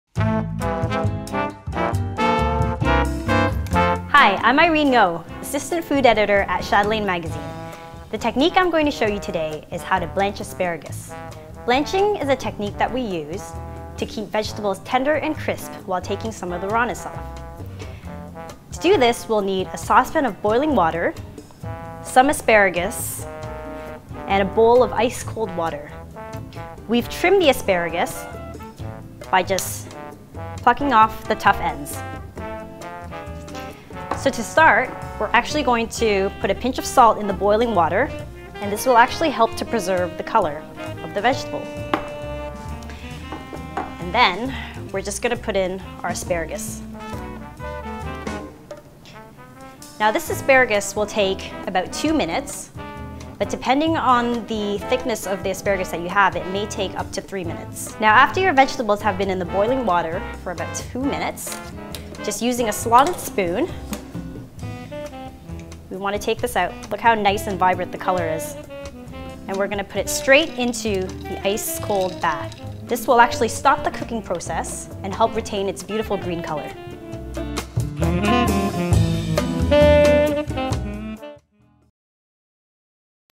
How to blanch asparagus
Like crisp veggies but want to take some of the rawness out?  Assistant food editor Irene Ngo shows you how.